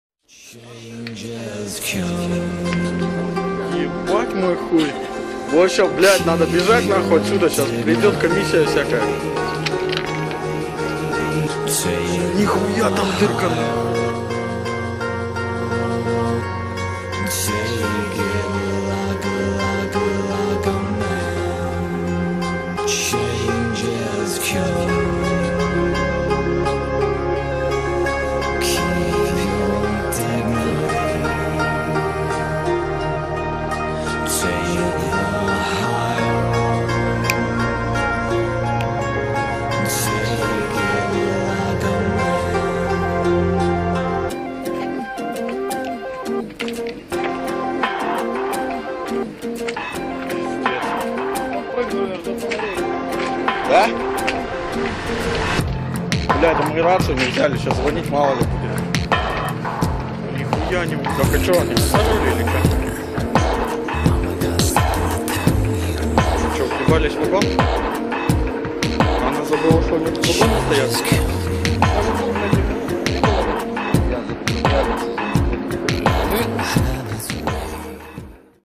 Крушение на станции Комбинатская. УЖДТ. Карельский Окатыш
Карелия. Костомукша. АО "Карельский Окатыш".  Северсталь. Костомукшский ГОК.
УЖДТ. Станция Комбинатская. 2014 год.
Сходы, аварии, крушения. Архив УЖДТ АО "Карельский Окатыш"